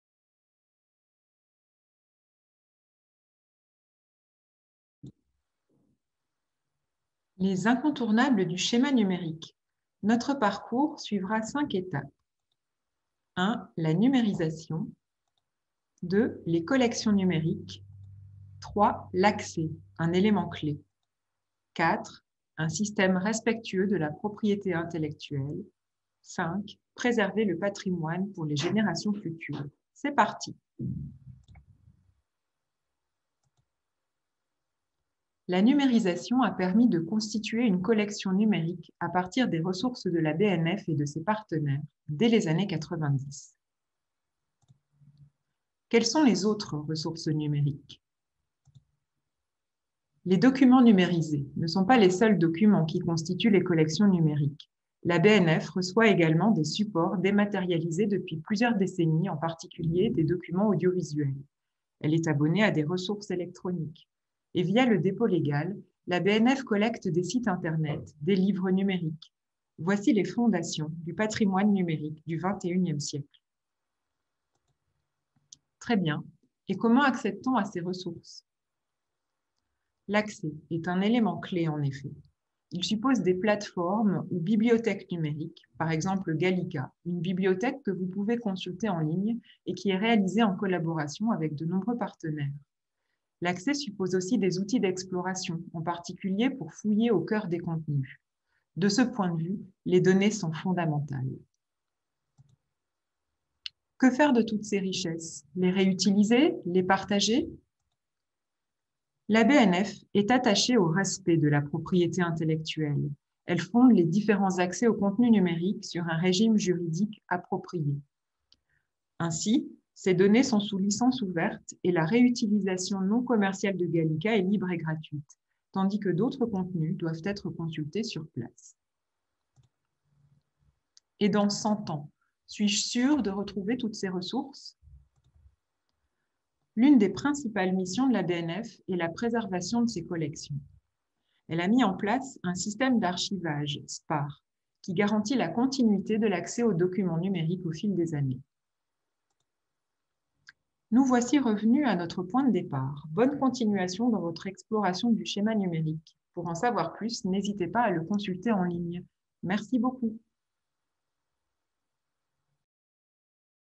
Parcours n° 1 : les incontournables du Schéma numérique
Vous ne savez pas par où commencer ? Vous ne connaissez pas la Bibliothèque nationale de France ? Ce premier parcours balise votre route de repères sûrs, qui vous permettront de mieux connaître les collections numériques de la Bibliothèque, les moyens d’y accéder, et la façon dont elles sont conservées.